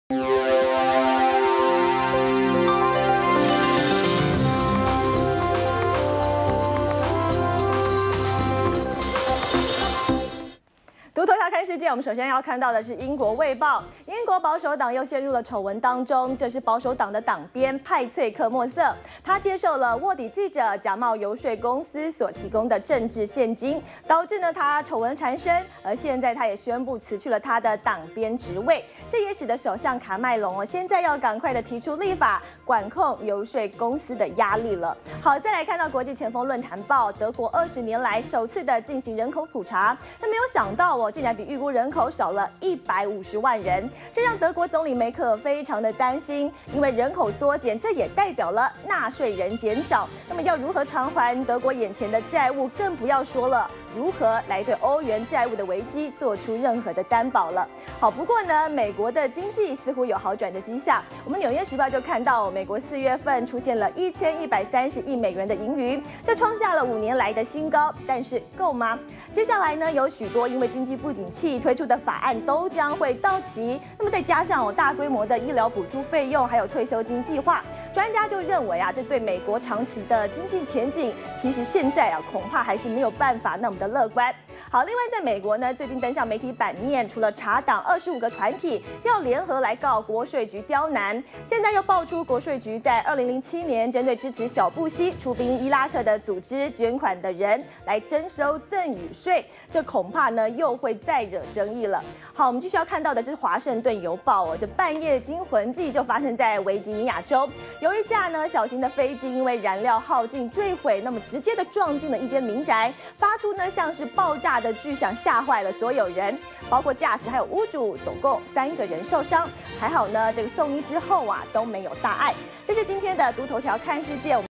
【2013.06.02】讀頭條看世界 -udn tv
udn早安新聞，帶您閱覽全球各大報紙頭條。
【英國衛報 陷政治獻金醜聞 保守黨黨鞭辭職】
【國際先鋒論壇報 德國人口普查 人口數下修150萬】
【紐約時報 美國財政轉盈餘 經濟前景仍堪憂】
【華爾街日報 共和黨組織捐贈者 國稅局關注】
【華盛頓郵報 美國飛機燃料耗盡 墜入民宅 】udn tv on FB Twitter G+